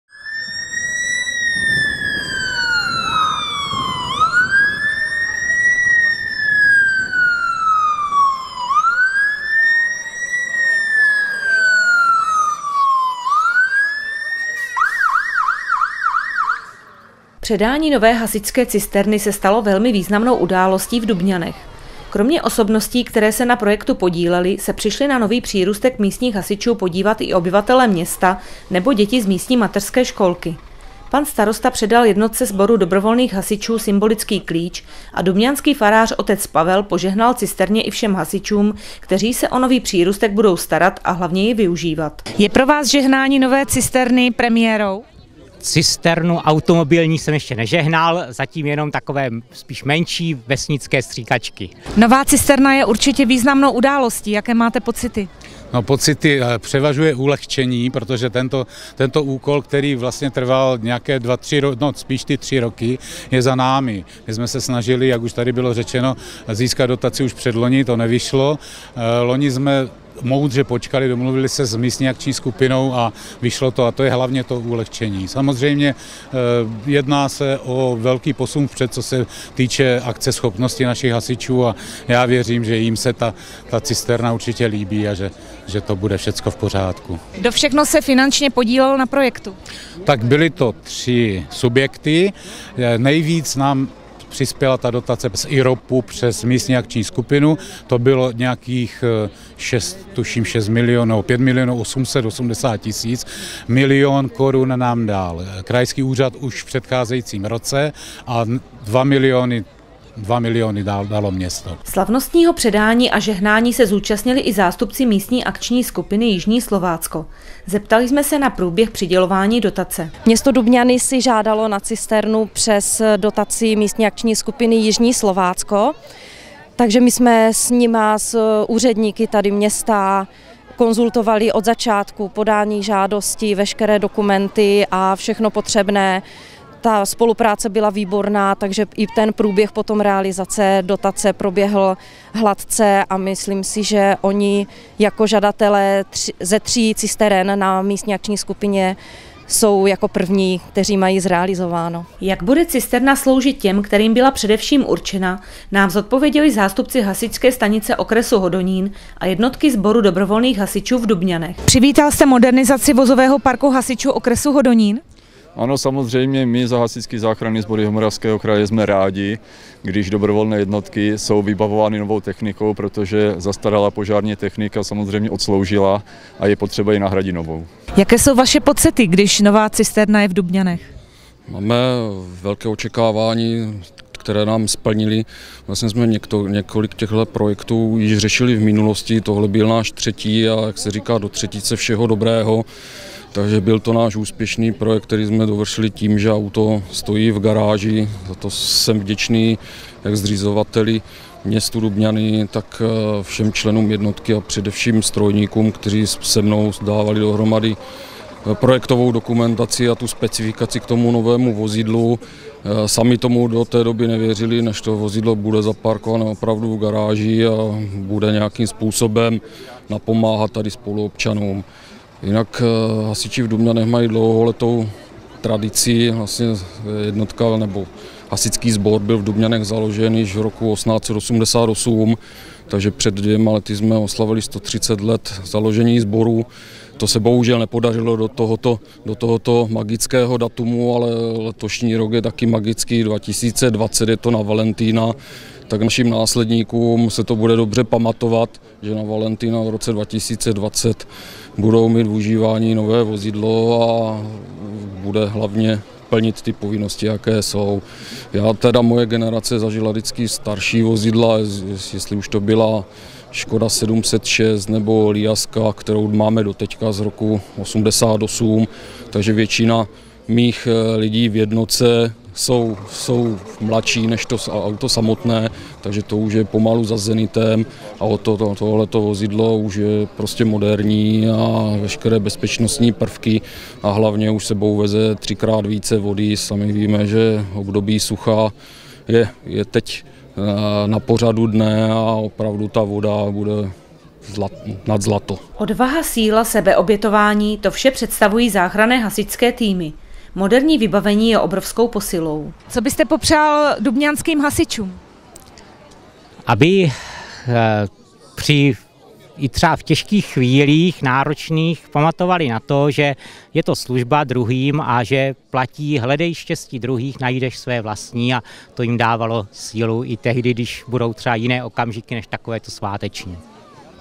Nové hasičské cisterně v Dubňanech požehnal Otec Pavel
Předání nové hasičské cisterny se stalo velmi významnou událostí v Dubňanech. Kromě osobností, které se na projektu podíleli, se přišli na nový přírůstek místních hasičů podívat i obyvatelé města, nebo děti z místní mateřské školky. Pan starosta předal jednotce sboru dobrovolných hasičů symbolický klíč a dubňanský farář Otec Pavel požehnal cisterně i všem hasičům, kteří se o nový přírůstek budou starat a hlavně jej využívat.